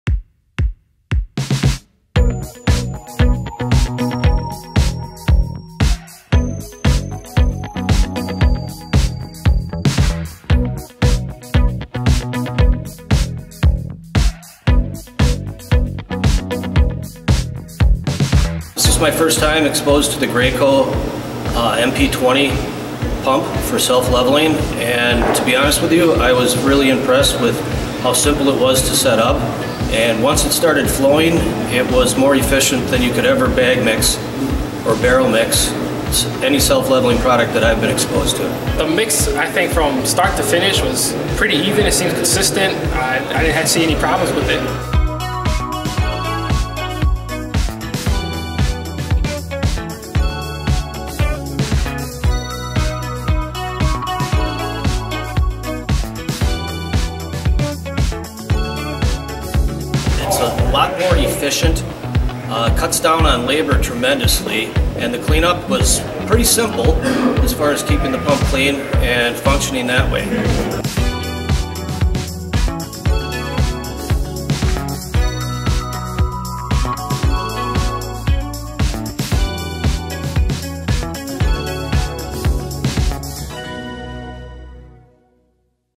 How to Install 2,200 sq. ft. of Self-Leveling in 33 Minutes
The Graco team visited The Southeast Wisconsin Carpentry Training Center to demonstrate how a ToughTek MP20 mixing pump will increase production without sacrificing quality. Trainers and apprentices took turns breaking bags of USG DUROCK™ BRAND QUIK-COVER™ self-leveling underlayment into the hopper while others pumped and raked the mixture into the 2,200 sq ft space. Within 33 minutes, the job was completed using 93 bags of material at 1/2" thick.